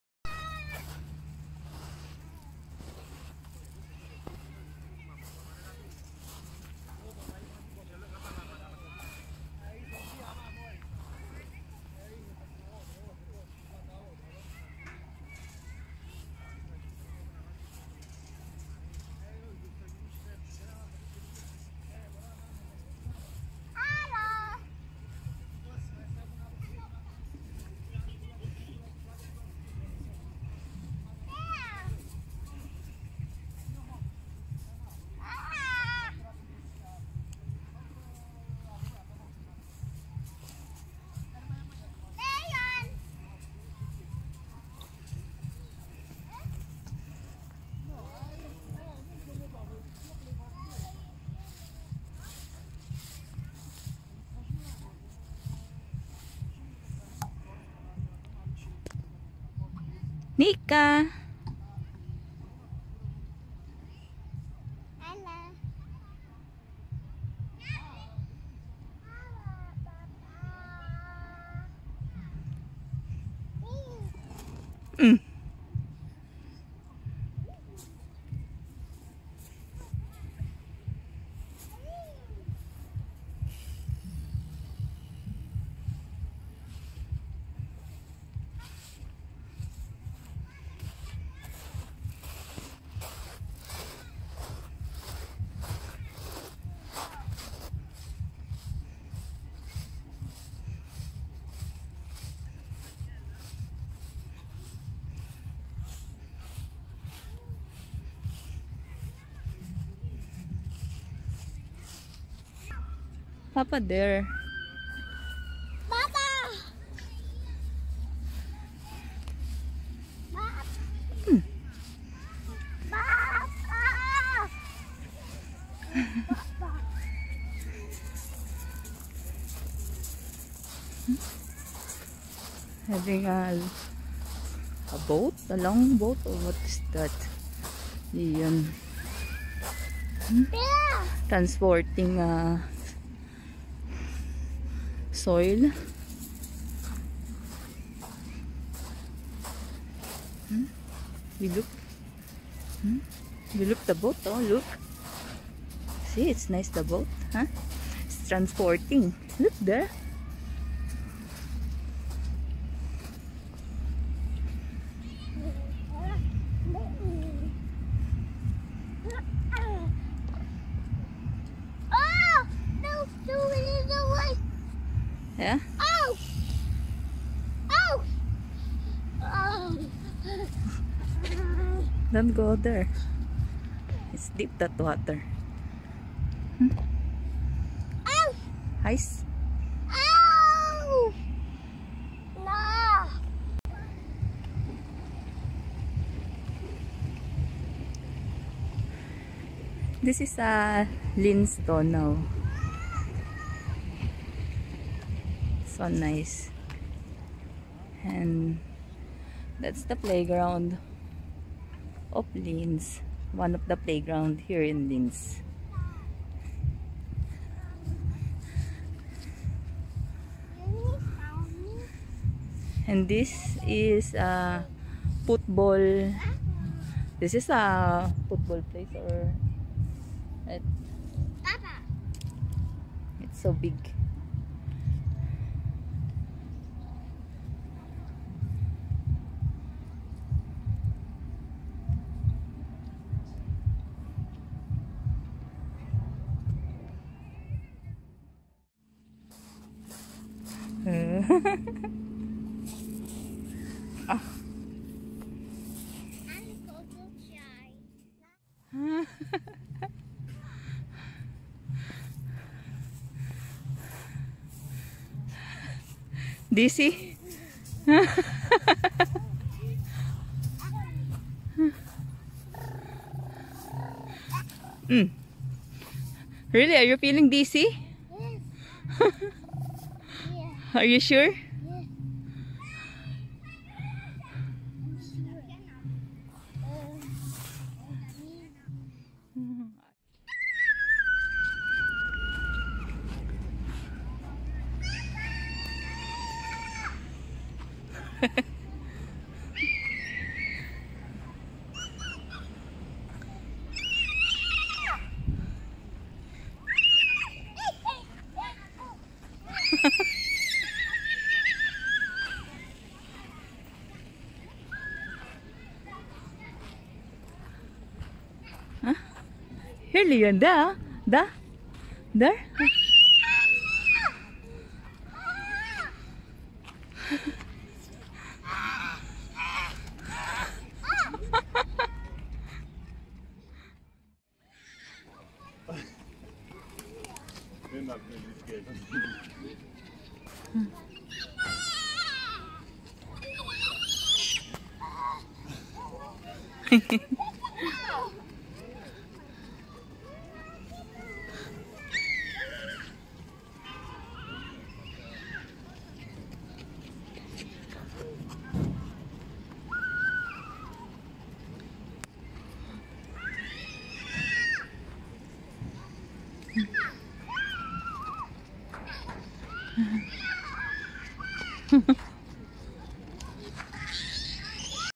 Playground in Linz, Austria
Exploring some playground in Linz one day is not enough my Kids having so much fun.. :